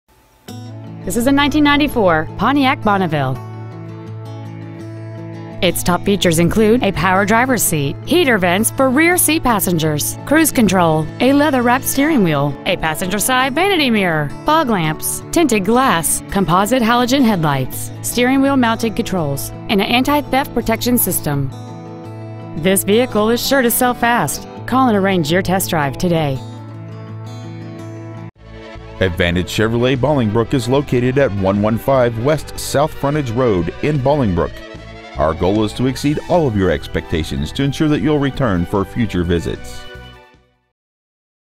Pre-Owned 1994 Pontiac Bonneville Chicago IL
We are proud to present this 1994 Pontiac Bonneville.

 Year: 1994
 Make: Pontiac
 Model: Bonneville

 Trans.: Automatic
 Exterior: Other
 Miles: 137,897
 Interior: Other

 115 W South Frontage Rd.

 Used 1994 Pontiac Bonneville  Bolingbrook IL